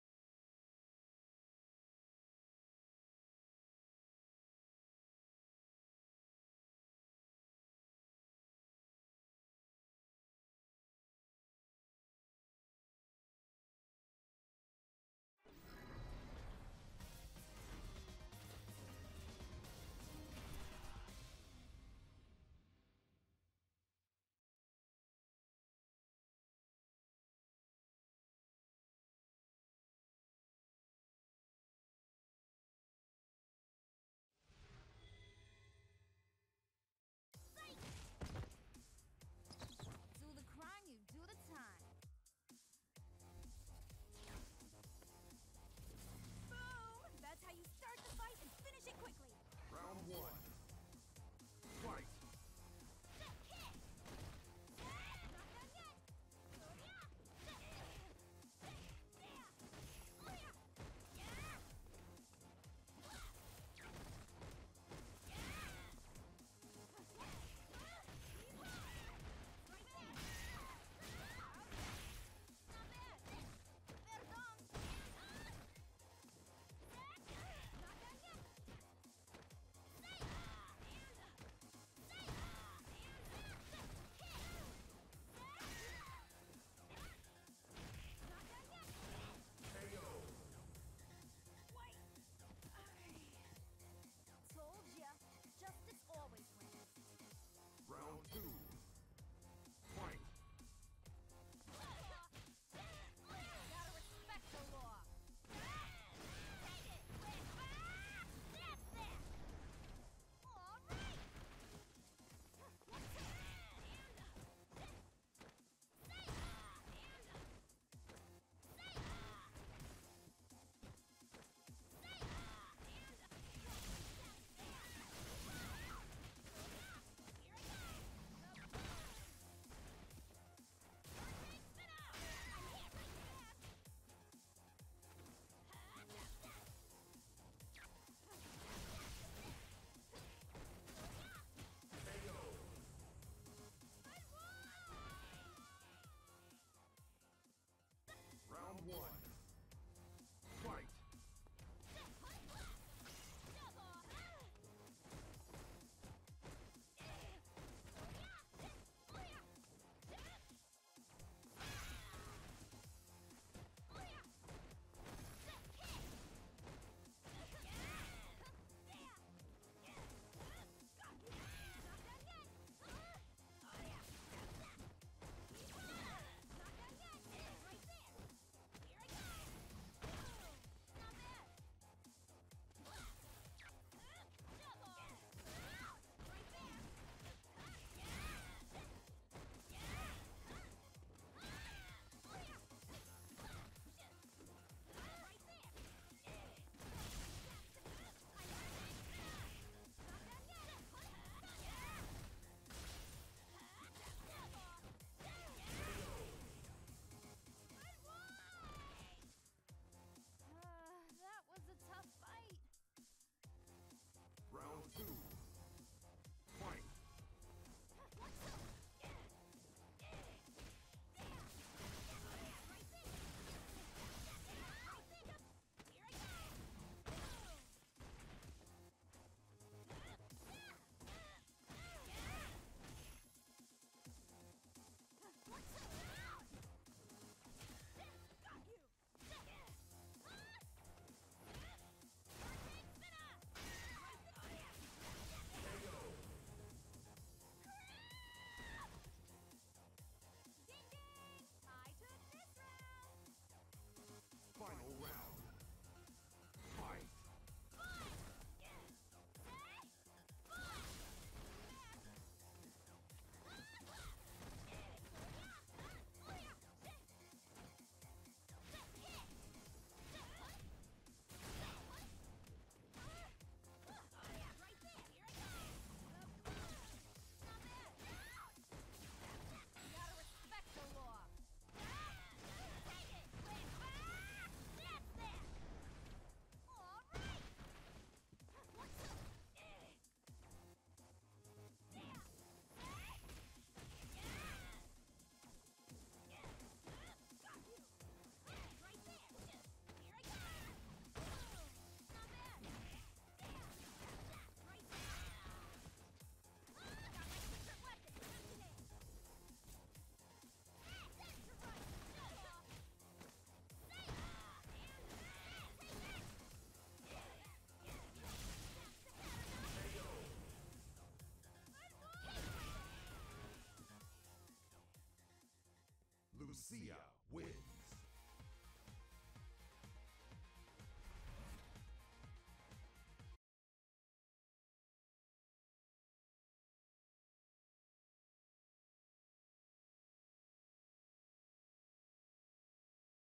Got my Platinum Rank back with Season 5 Lucia
Played some ranked matches. Broke back through to Platinum.

Not my best match but adapted well and came out on top.
Not seen in the video was the rocky connection between us during the match.